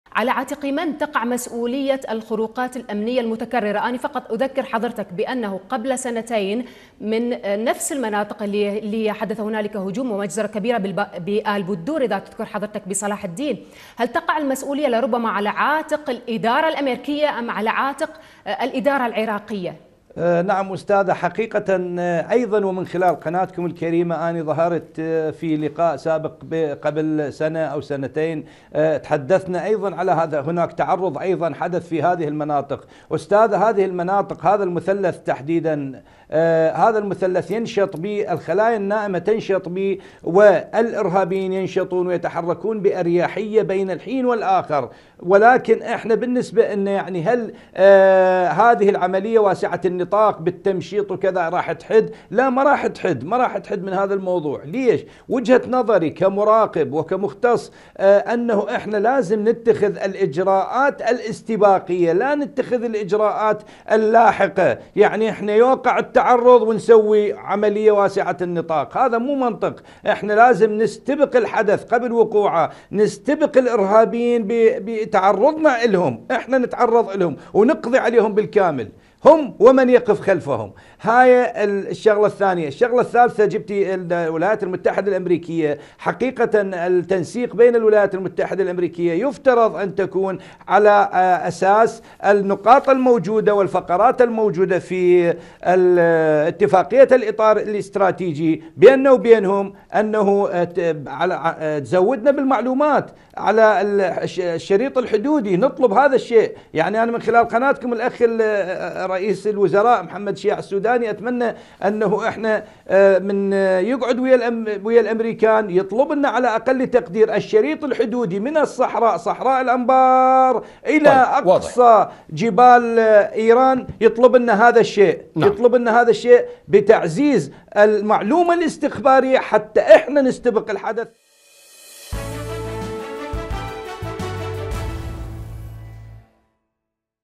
بعد هجوم مطيبيجة.. من يتحمل مسؤولية الكارثة ؟!
موقع قناة التغيير الفضائية  الالكتروني